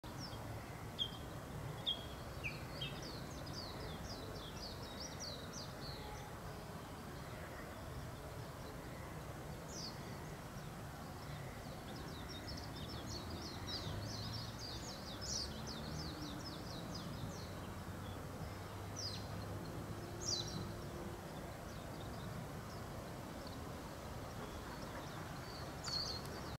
4Kハンディカムで桜とウメジロー
咲き始めた桜に　メジロが蜜を吸いにきました